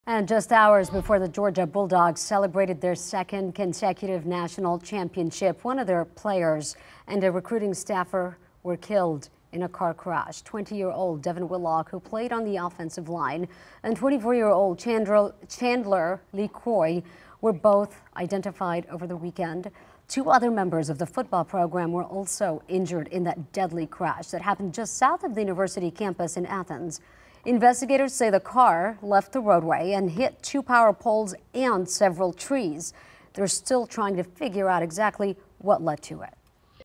UGA player, recruiter killed in car wreck
Devin Willock, 20, who played on the offensive line and Chandler Lecroy, 24, were both identifed over the weekend.